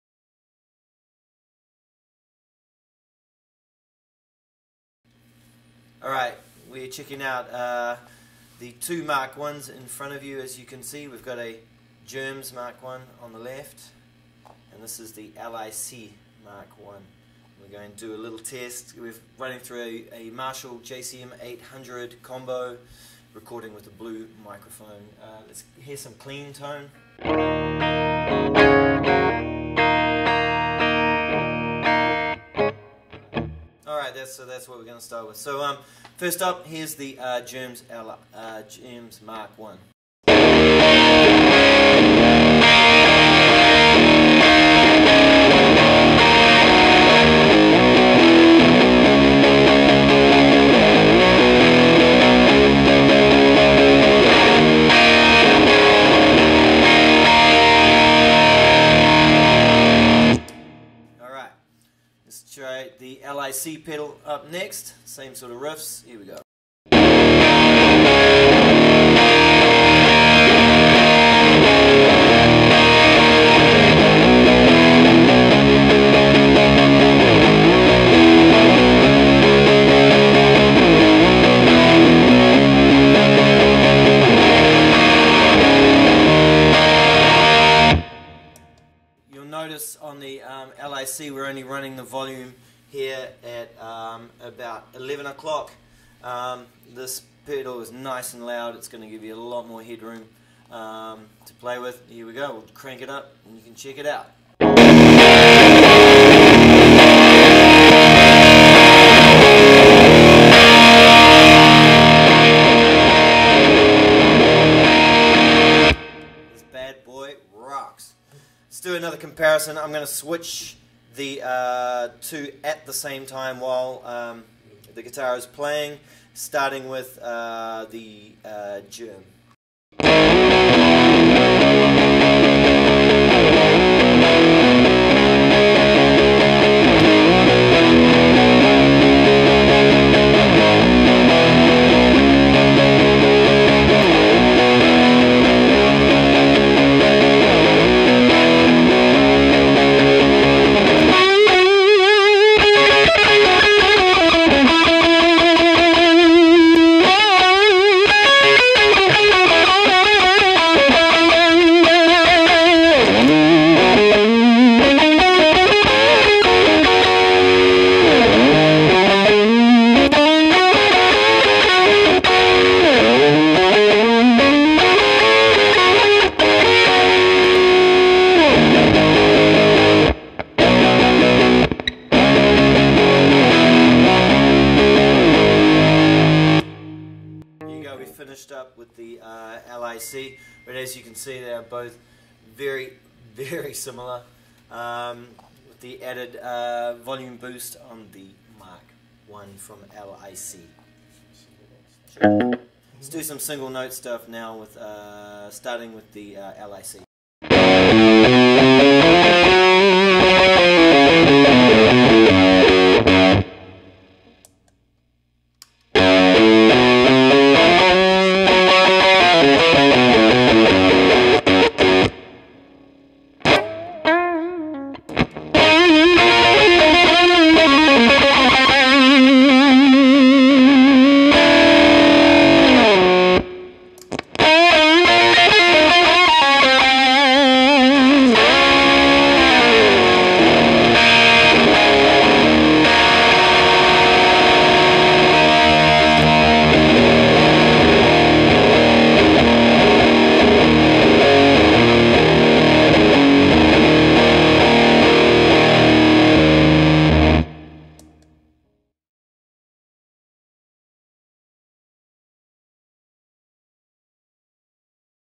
Jerms MKI vs. LIC Pedals MKI
Comparison between what many believe to be the best MKI available, The Jerms & LIC Pedals MKI. Recorded through a JCM 800 2 X 12 Combo, Les Paul, Sparks Blue Mic into Garage Band.